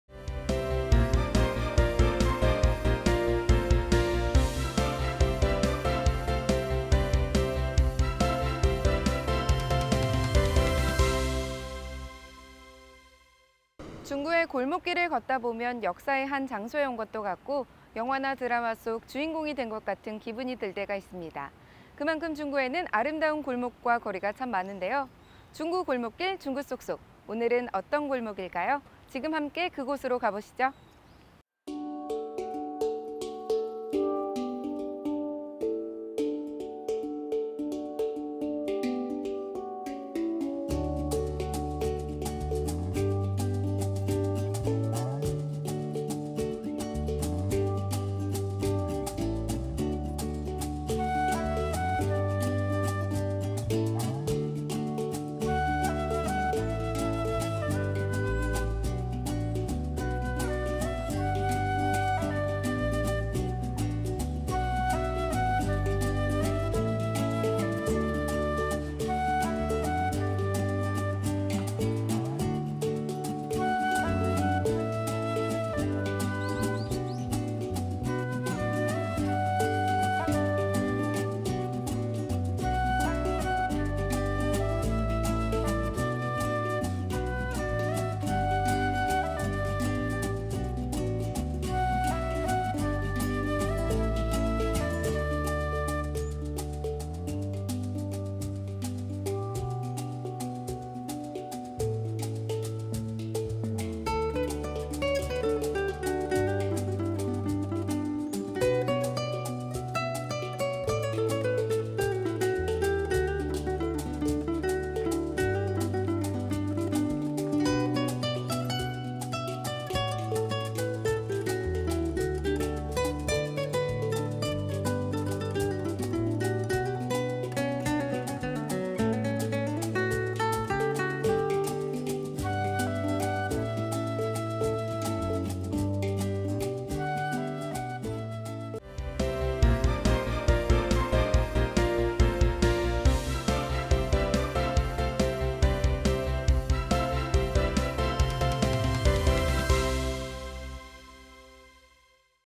[인천중구TV 뉴스] 중구 속속 - 조병수 가옥
대한제국시대 유적건조물이자, 1997년 문화재자료 제16호로 지정된 곳!
백 년을 넘게 살아 숨 쉬는 옛 가옥이 궁금하다면?
역사 문화도시 중구로 오세요~